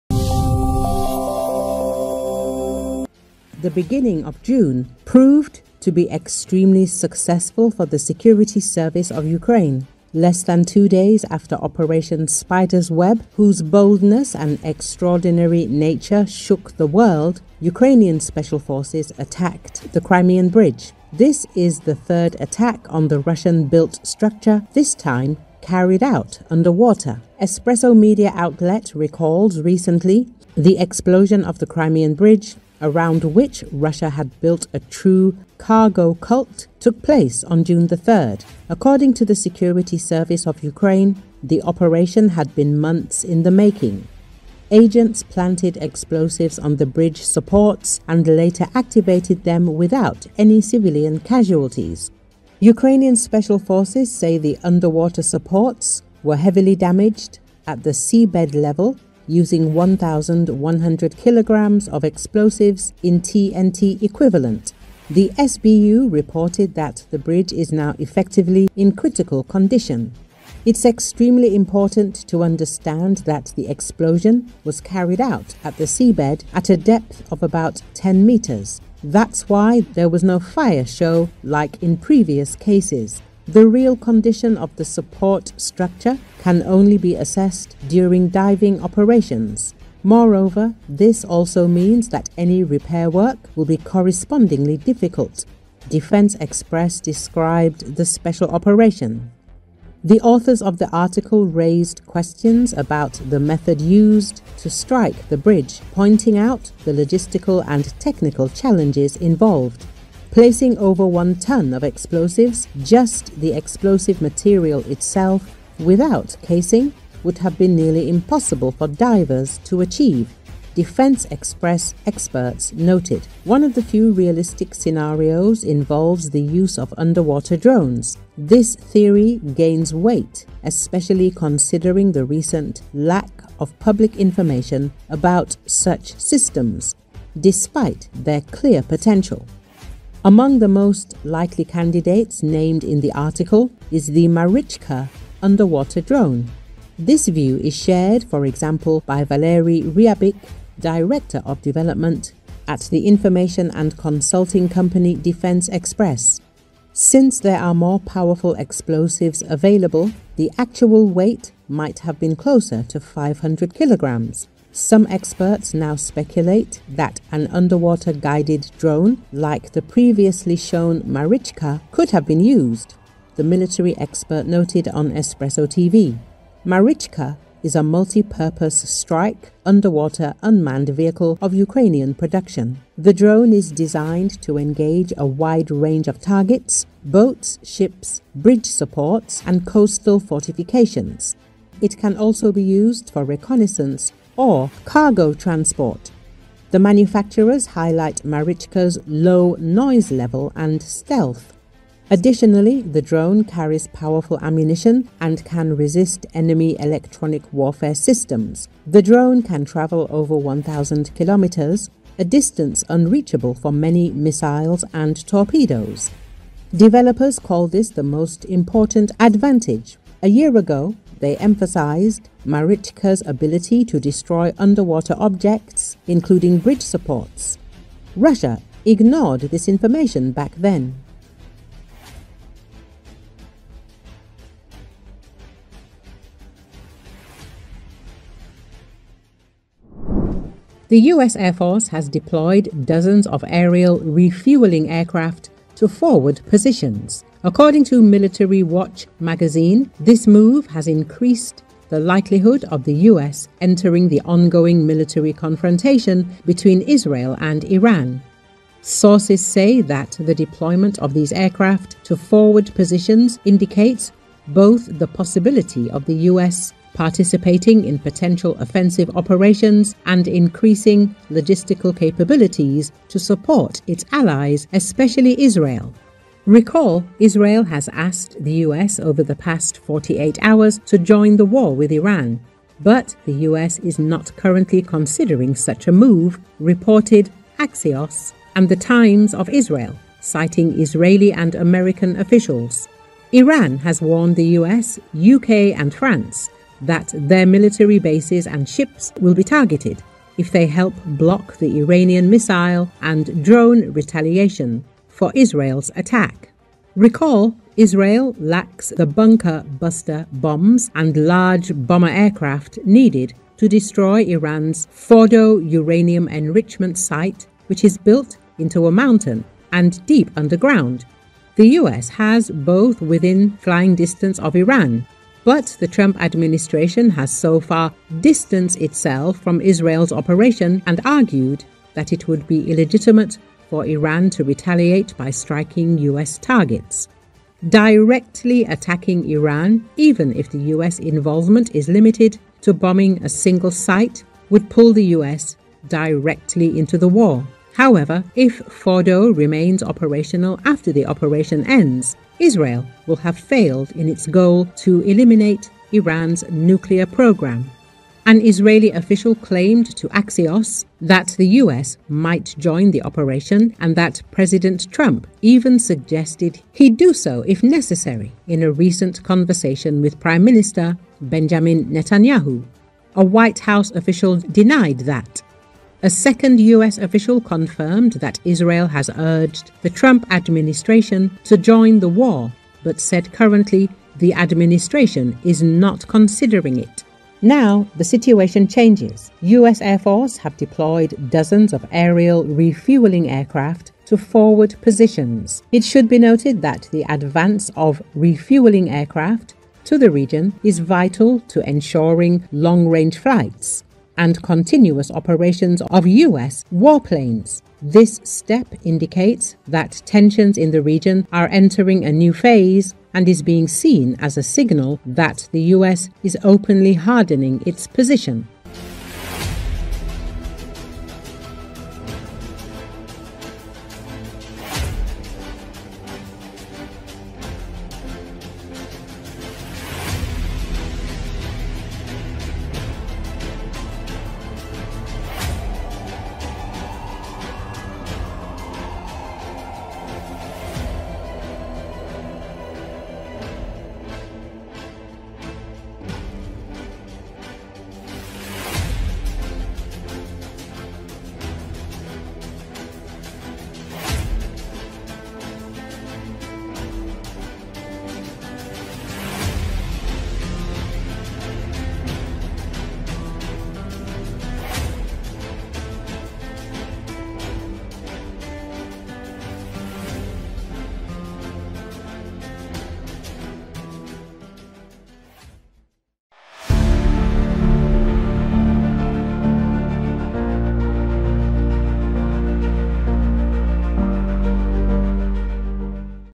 Ukraine’s Marichka underwater drones vs Russian torpedos; Russian fleet are targeted by these drones
The beginning of June proved to be extremely successful for the Security Service of Ukraine. Less than two days after Operation Spider’s Web, whose boldness and extraordinary nature shook the world, Ukrainian special forces attacked the Crimean Bridge. This is the third attack on the Russian-built structure, this time carried out underwater.

Espresso media outlet recalls, recently,the explosion of the Crimean Bridge, around which Russia had built a true cargo cult, took place on June 3. According to the Security Service of Ukraine, the operation had been months in the making. Agents planted explosives on the bridge supports and later activated them without any civilian casualties.

Ukrainian special forces say the underwater supports were heavily damaged at the seabed level using 1,100 kg of explosives in TNT equivalent. The SBU reported that the bridge is now effectively in critical condition.

“It’s extremely important to understand that the explosion was carried out at the seabed, at a depth of about 10 meters. That’s why there was no fire show like in previous cases. The real condition of the support structure can only be assessed during diving operations. Moreover, this also means that any repair work will be correspondingly difficult,” Defense Express described the special operation.

The authors of the article raised questions about the method used to strike the bridge, pointing out the logistical and technical challenges involved.

“Placing over one ton of explosives — just the explosive material itself, without casing — would have been nearly impossible for divers to achieve,” Defense Express experts noted. “One of the few realistic scenarios involves the use of underwater drones. This theory gains weight especially considering the recent lack of public information about such systems, despite their clear potential.”

Among the most likely candidates named in the article is the Marichka underwater drone. This view is shared, for example, by Valerii Riabykh, Director of Development at the information and consulting company Defense Express. “Since there are more powerful explosives available, the actual weight might have been closer to 500 kg. Some experts now speculate that an underwater guided drone, like the previously shown Marichka, could have been used,” the military expert noted on Espreso TV. Marichka is a multipurpose strike underwater unmanned vehicle of Ukrainian production. The drone is designed to engage a wide range of targets — boats, ships, bridge supports, and coastal fortifications. It can also be used for reconnaissance or cargo transport. The manufacturers highlight Marichka’s low noise level and stealth. Additionally, the drone carries powerful ammunition and can resist enemy electronic warfare systems. The drone can travel over 1,000 km, a distance unreachable for many missiles and torpedoes.         Developers call this the most important advantage. A year ago, they emphasized Marichka’s ability to destroy underwater objects, including bridge supports. Russia ignored this information back then.